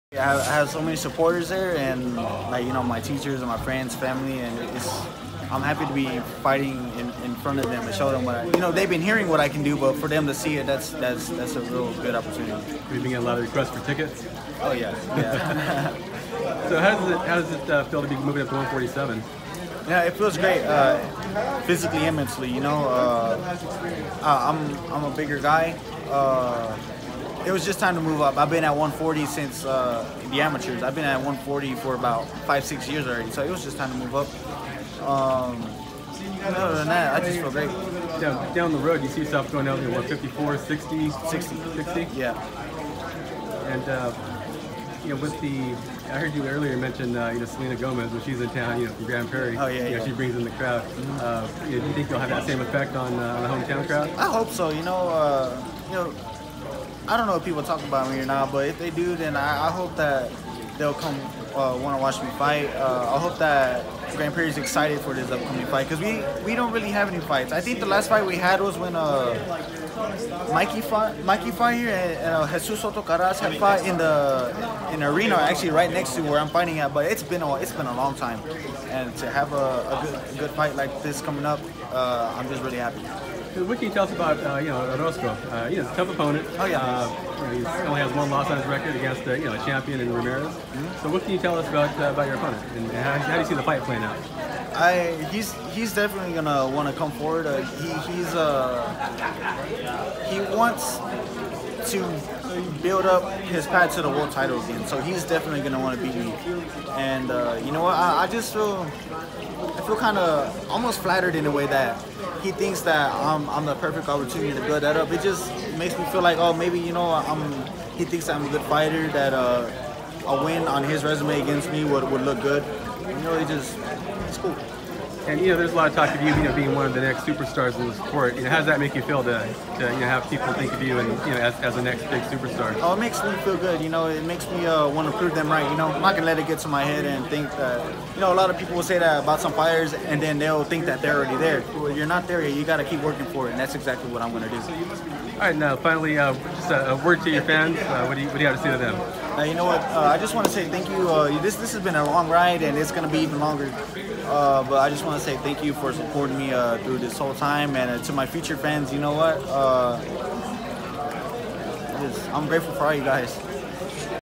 Vergil Ortiz Jr talks about his homecoming fight and moving up to 160
Vergil Ortiz press conference at El Cholo Restaurant in Los Angeles, CA